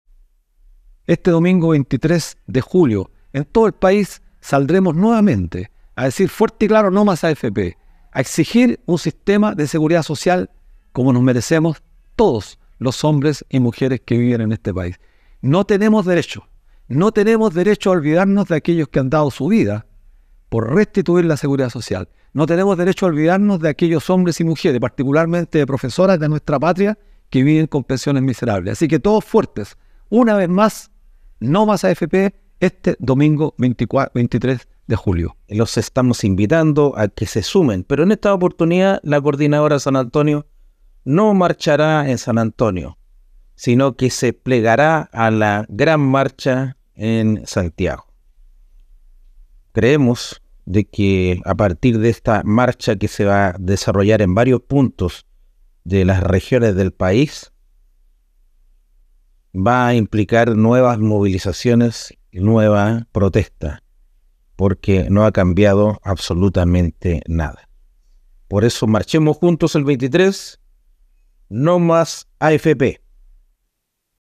21 JUL 2023 Coordinadora No Mas AFP vuelve a las calles este domingo 23 de julio
Coordinadora no Más AFP de San Antonio se suma a la movilización nacional de este domingo 23 de julio. En esta oportunidad en la comuna de San Antonio no se realizará marcha, pero si los representantes de la coordinadora local, se unirán a la manifestación en la región metropolitana. Coordinadora No Más AFP vuelve a las calles para recordar los 7 años de llamado de atención hacia las autoridades pertinentes, para exigir terminar con el actual sistema de pensiones. Quienes se refieren a esta convocatoria son Luis Mesina, vocero nacional y Sergio Castro vocero de San Antonio de la Coordinadora No Mas AFP.